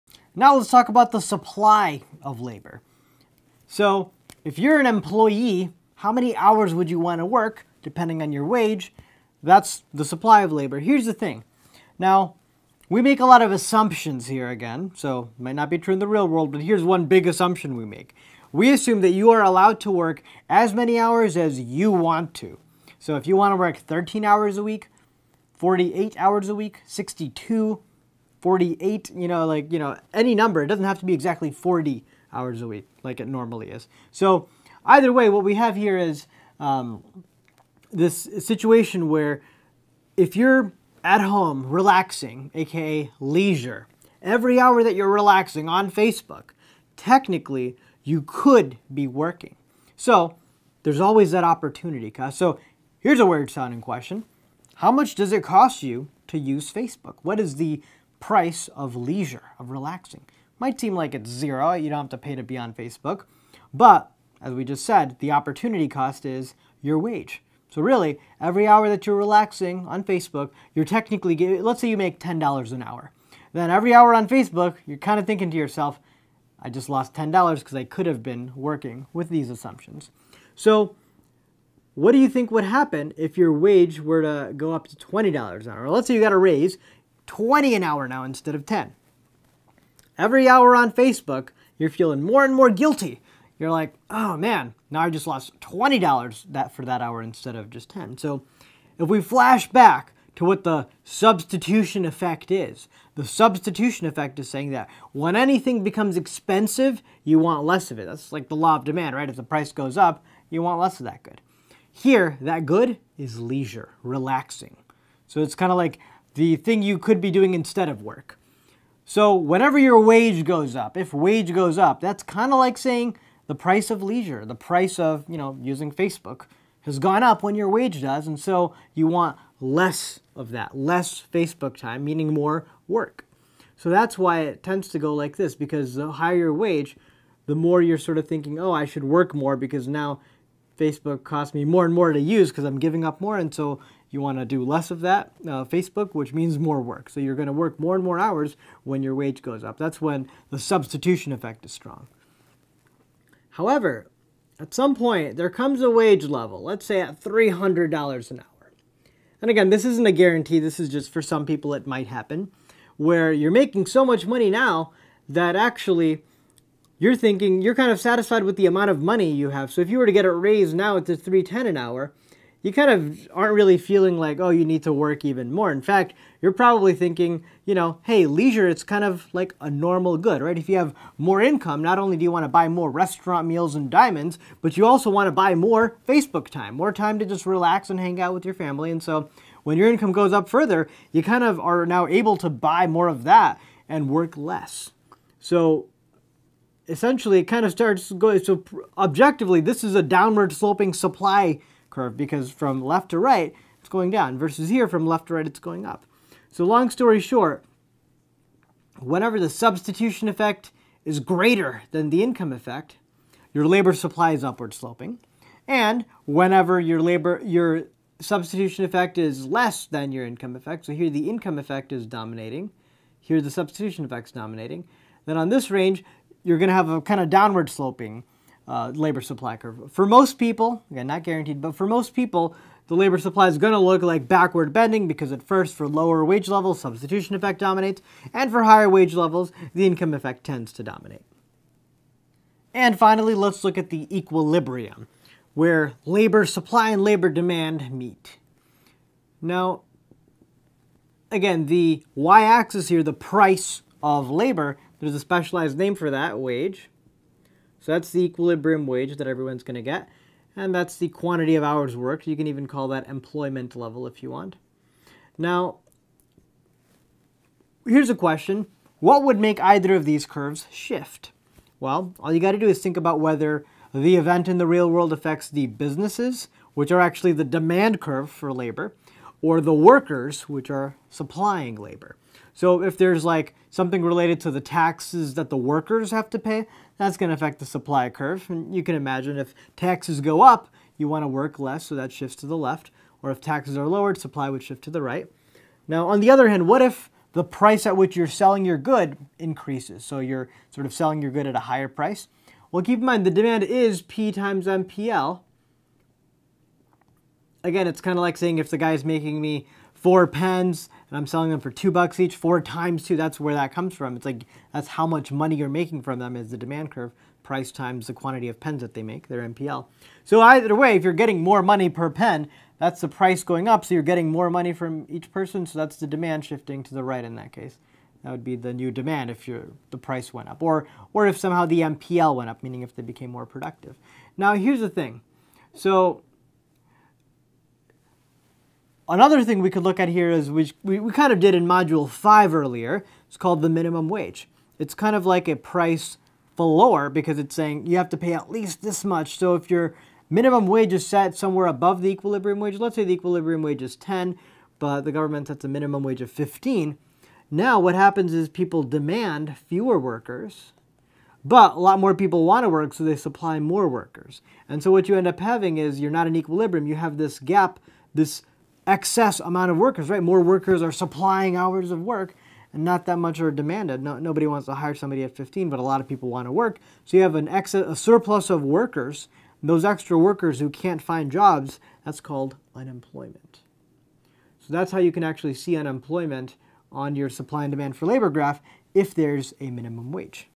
"Backward Bending" Labor Supply
Rohen Shah explains the Labor Supply - "Backward Bending" - as well as equilibrium and unemployment.